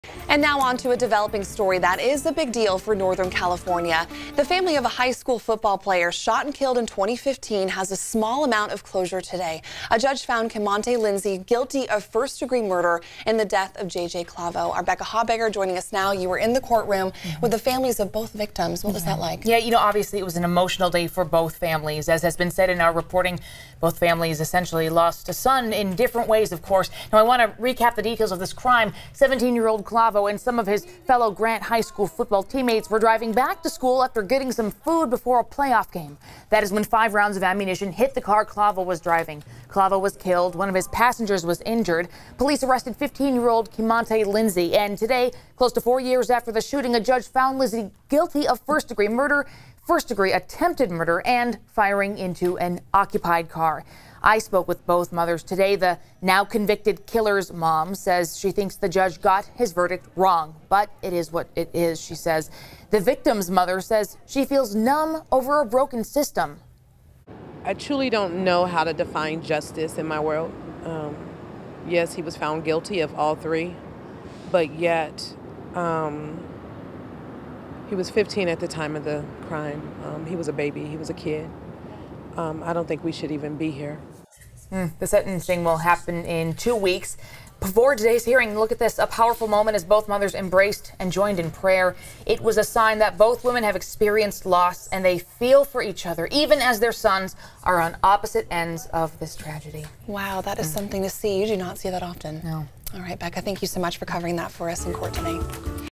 Guilty verdict returned in murder of Grant Union High School student JJ Clavo
The teen accused of killing high school football player Jaulon "JJ" Clavo in 2015 was found guilty Monday. Keymontae Lindsey faced three charges: first-degree murder with a firearm, first-degree attempted murder with a firearm and firing a firearm into an occupied motor vehicle. He was convicted on all counts.